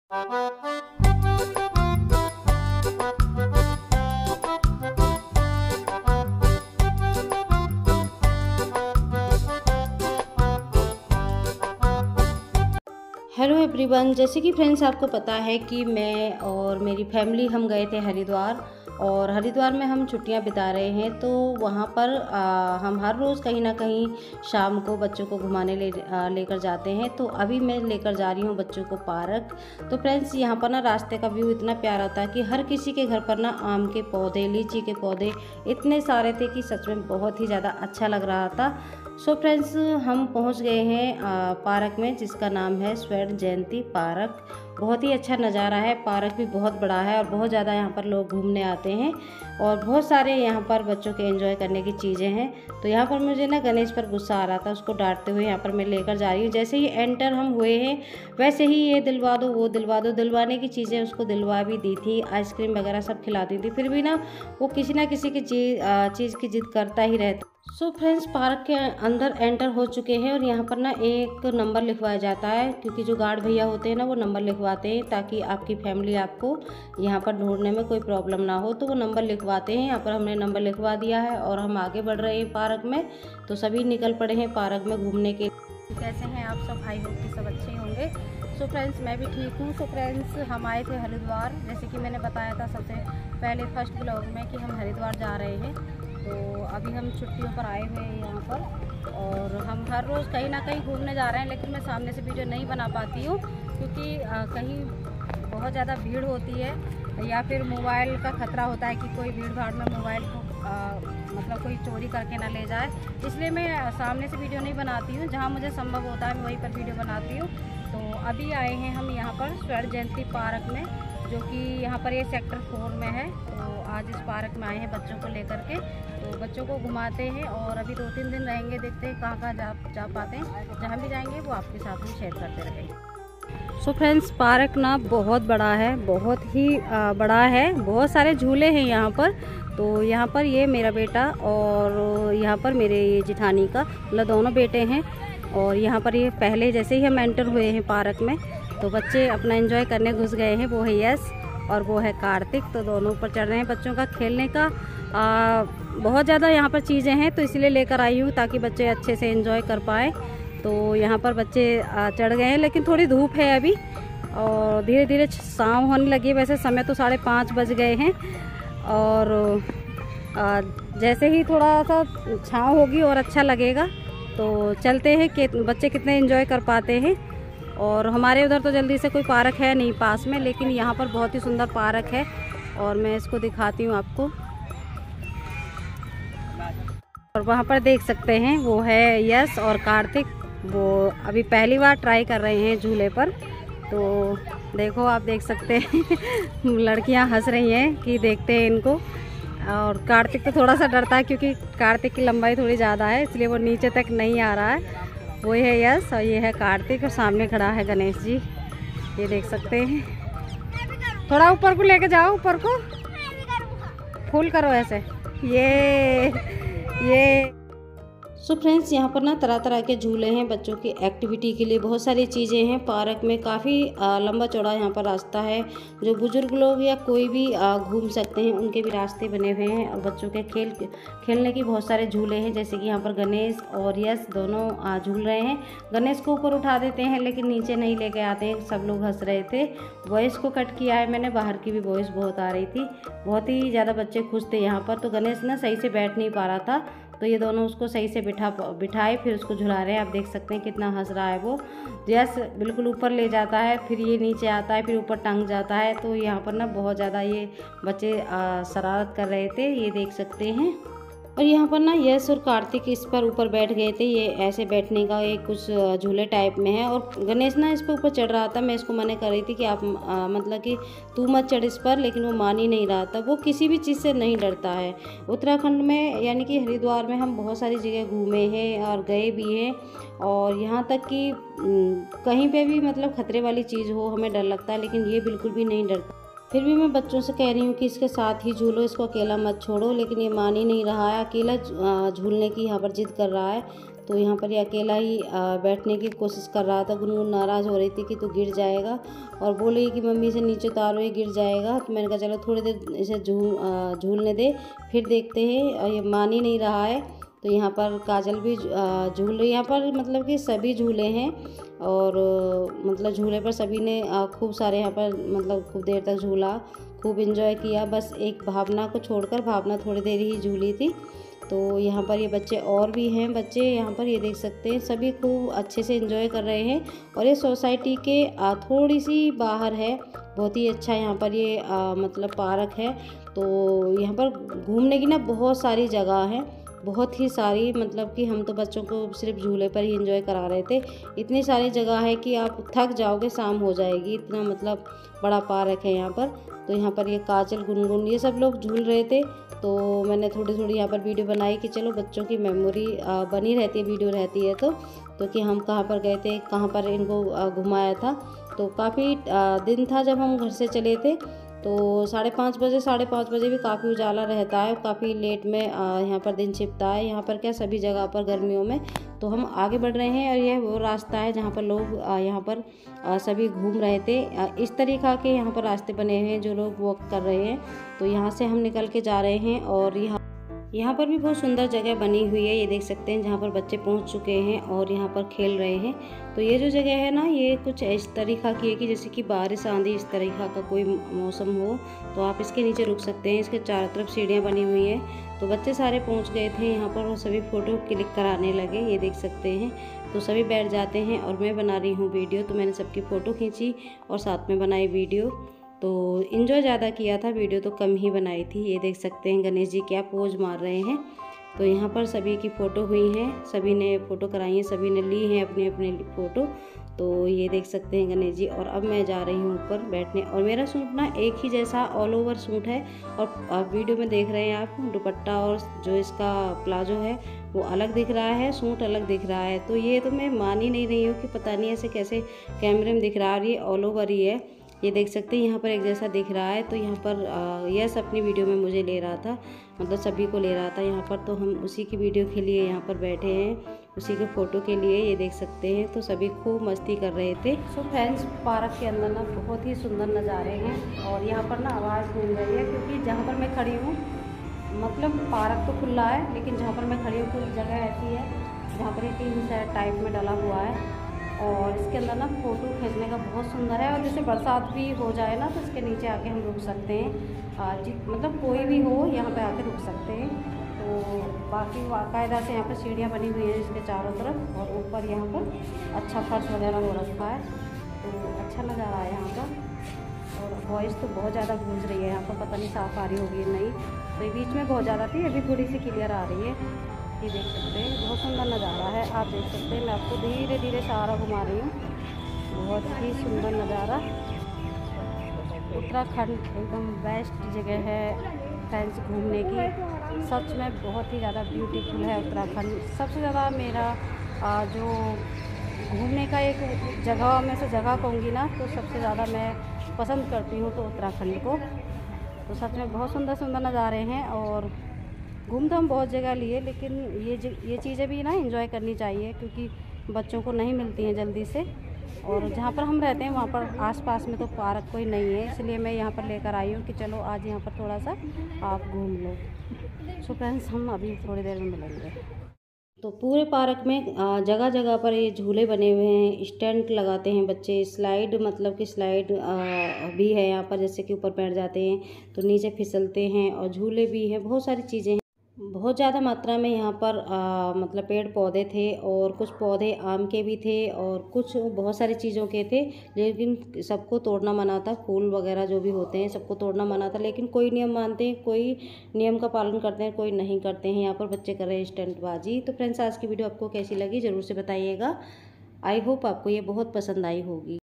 हद है मेरे बेटे से 🫣 झूले से उतरना भी नहीं आता 😂 फुल मस्ती कि हम सभी लोगों ने
हद है मेरे बेटे से 🫣 झूले से उतरना भी नहीं आता 😂 फुल मस्ती की सभी ने Swarnajayanti parkHaridwar